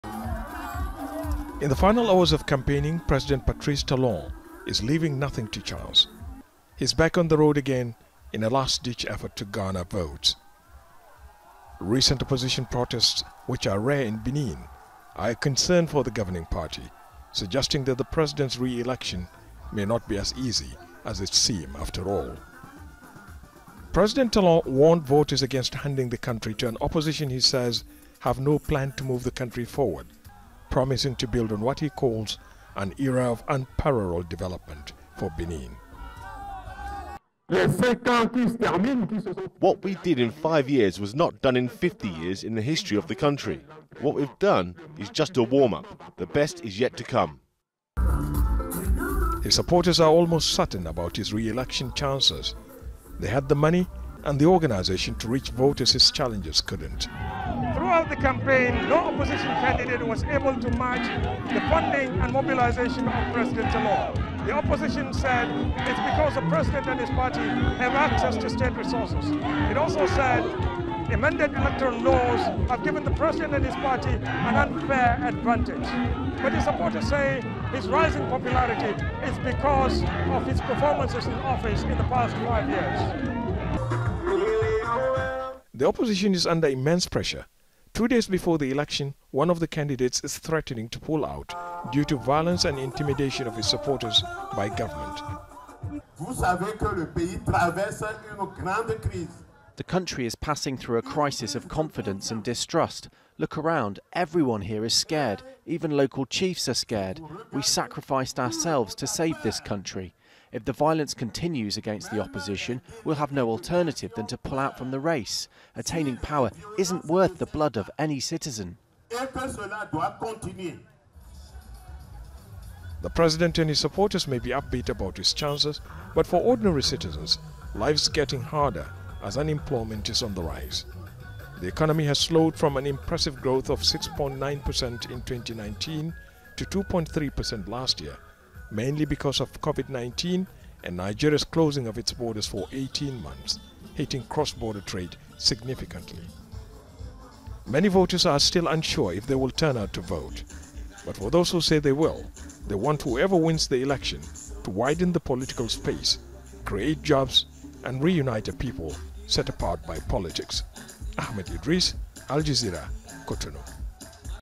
Benin election: Campaigning ends ahead of presidential vote
Campaigning has ended in Benin ahead of Sunday's presidential election.
President Patrice Talon is facing a weakened opposition in a vote he has expected to win by a landslide.
Most of his rivals have been silenced or forced into exile.
Al Jazeera's Ahmed Idris reports from Benin's commercial capital, Cotonou.